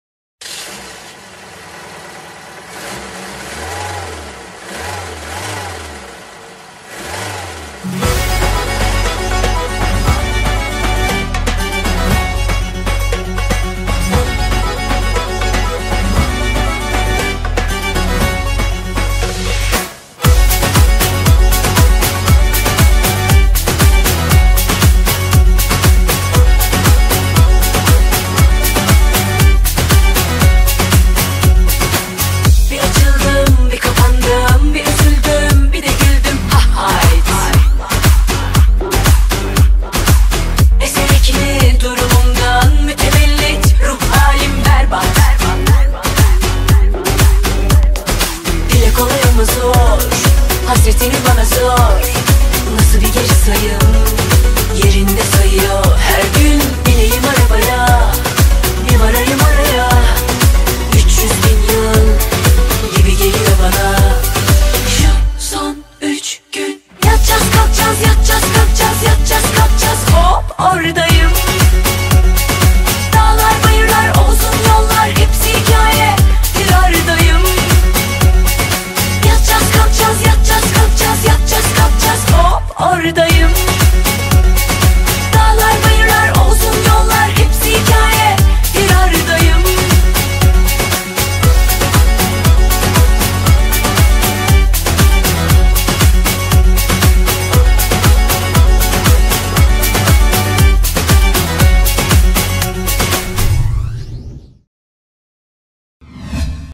Iceberg Events Presents Gulsen Live in Concert Official Aftermovie HD
Iceberg Events Presents Gulsen Live in Concert Official Aftermovie | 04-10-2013 | Amsterdam Convention Factory